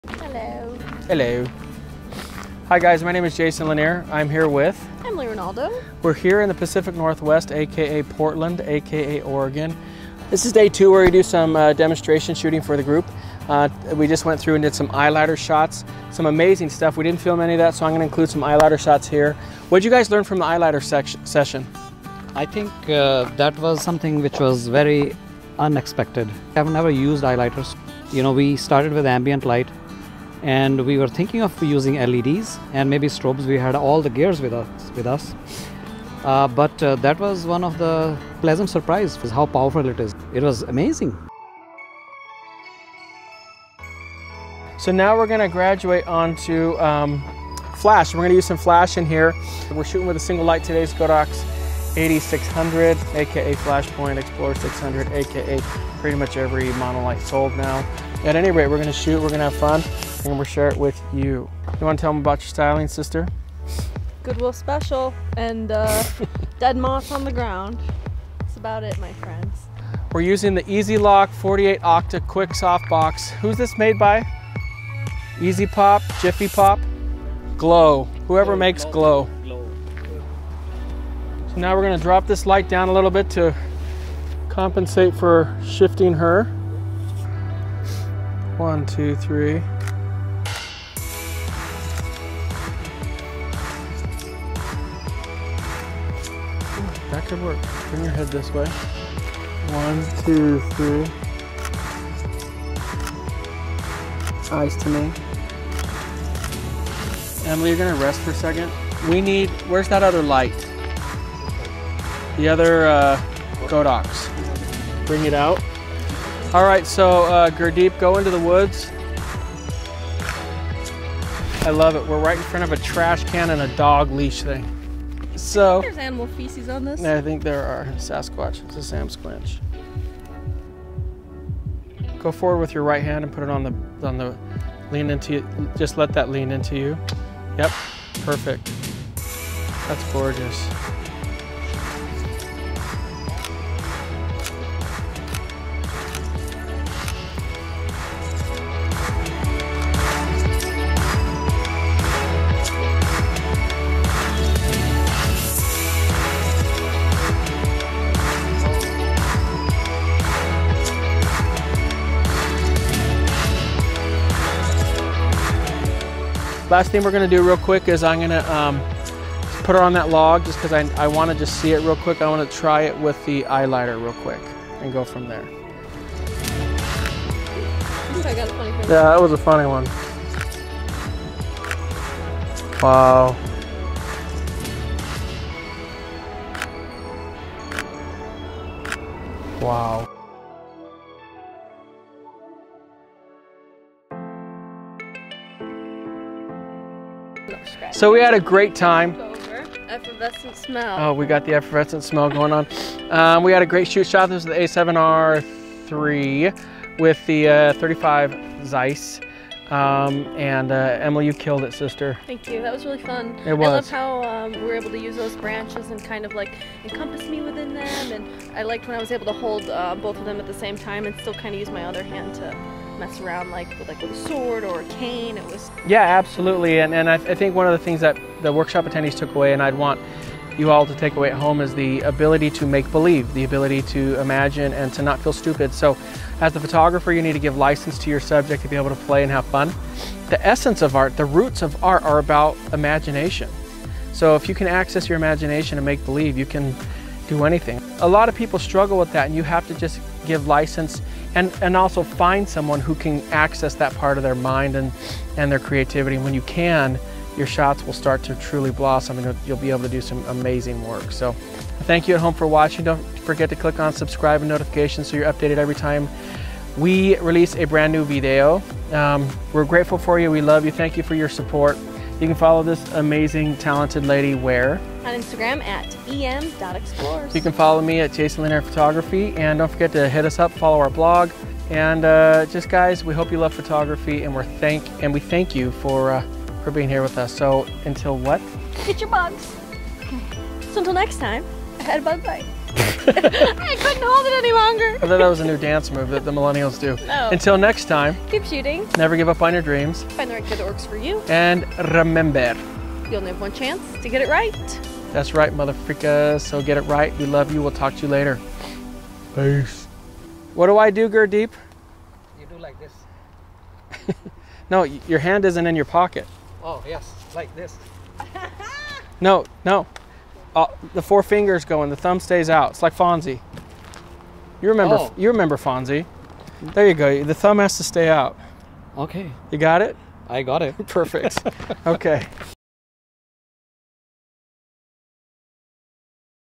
Using Flash Photography and Reflectors at a Jason Lanier Photography Workshop Video
Join Jason Lanier at his workshop conducted in Portland, Oregon where he shows you how he uses flash photography for some pretty cool results.

In this shoot Jason uses the Sony A7Riii with the Sony Zeiss 50mm lens.  For lighting he uses the Godox AD600 Pro with the Glow EZ Lock Octabox.  He also includes images that he took using the Westcott Eyelighter for demonstration purposes as well.

-Sony A7Riii

-Sony Zeiss 50mm

-Godox AD600Pro

-Glow EZ Lock

We hope you enjoy the video and invite you to stick around and watch a few more while you're here.

Thanks,

Jason Lanier Photography